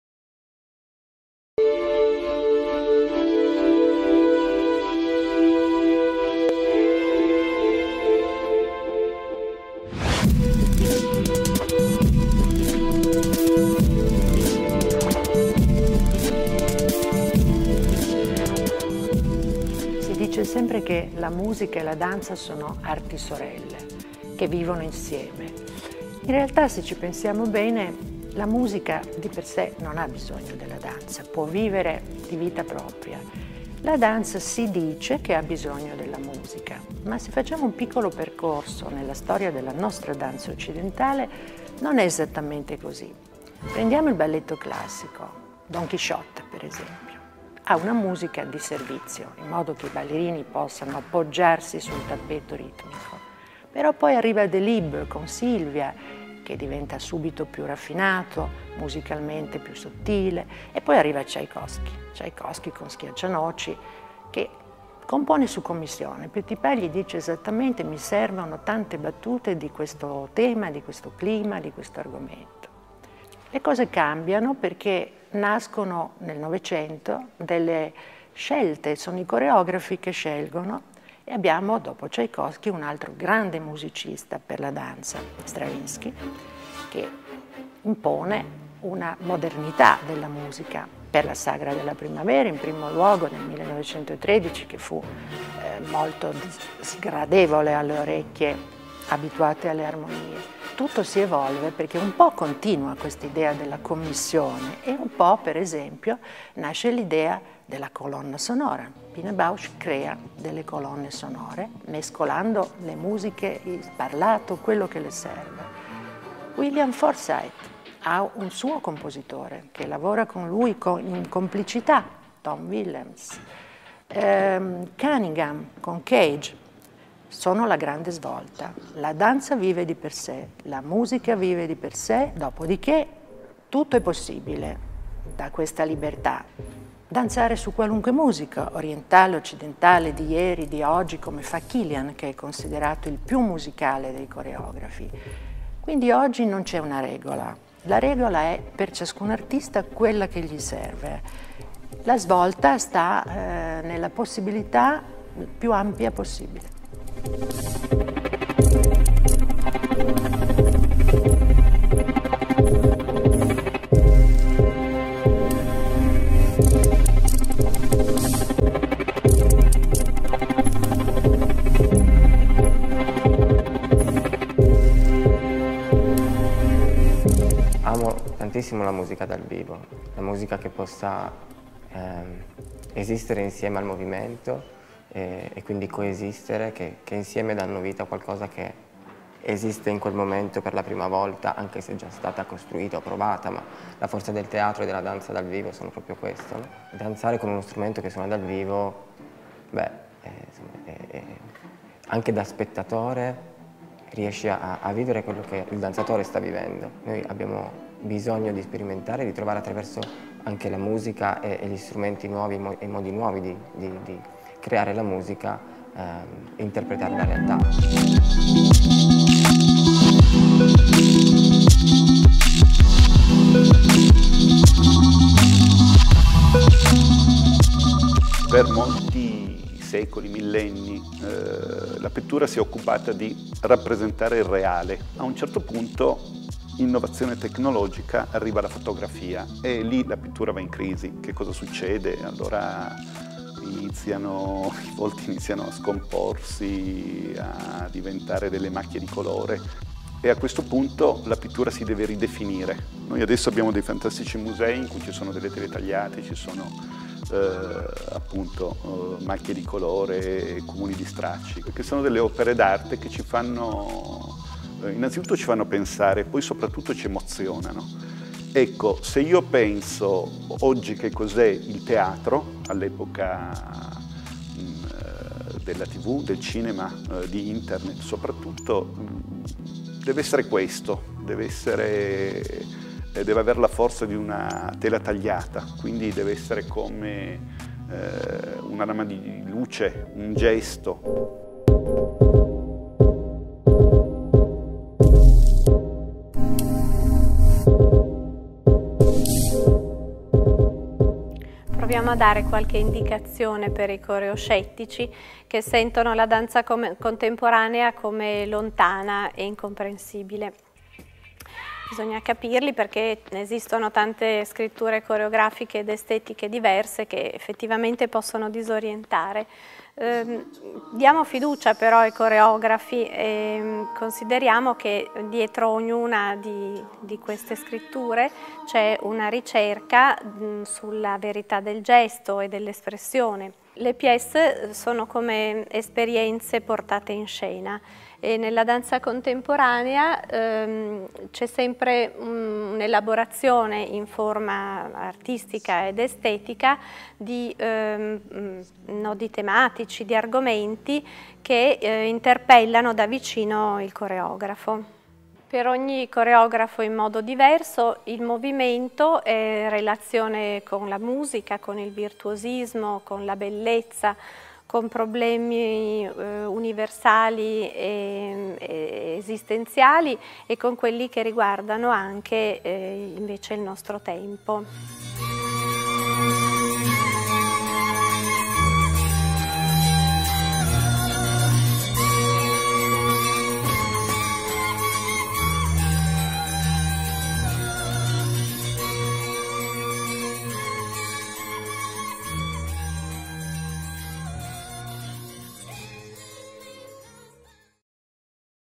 Il gesto Musicale
Danza: PIccolo Manuale d'uso
Interventi di Elisa Guzzo Vaccarino / Daiele Ninarello / Chiara Castellazzi / Roberto Tarasco
Video a Cura di Chiara Pacilli

Danza: piccolo manuale d'uso è una serie di tre incontri che si svolgeranno presso il Circolo dei lettori (Palazzo Graneri della Roccia -- Via Bogino, 9 -- Torino) il 7, il 14 e il 21 maggio 2013, alle ore 18.00, organizzati dal Circolo dei lettori in collaborazione con Torinodanza Festival e la Fondazione del Teatro Stabile di Torino. Nel corso di questi incontri saranno affrontati tre diversi temi: Il corpo magico, Il gesto musicale e Una strana bellezza. Ciascun tema sarà presentato con brevi interventi dal vivo e con la proiezione di video realizzati ad hoc e ci sarà per ogni incontro un rimando diretto ad alcuni dei principali appuntamenti di Torinodanza Festival che andrà in scena in autunno, dal 13 settembre al 9 novembre 2013. Un ciclo di approfondimento che troverà la sua naturale e ideale collocazione sul sito web di Torinodanza